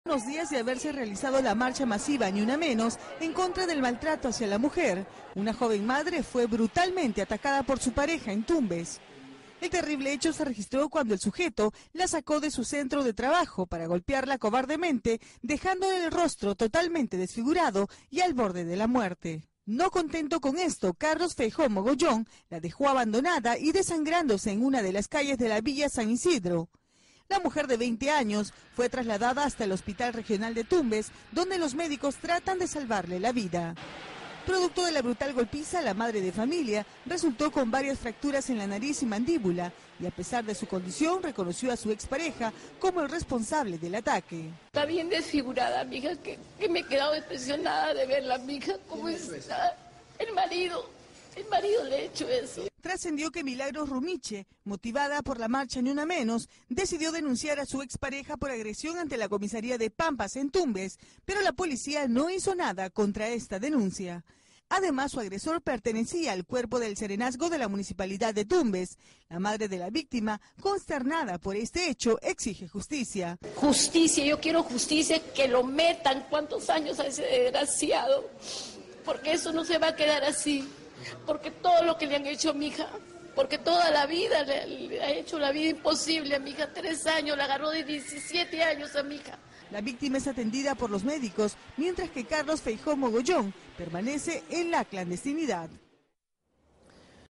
INDIGNANTE: sujeto masacra a su expareja porque lo denunció en comisaría
Tumbes: motivada por la marcha 'Ni Una Menos', joven denunció a su pareja pero agresor no fue detenido. Ahora la víctima está luchando por su vida.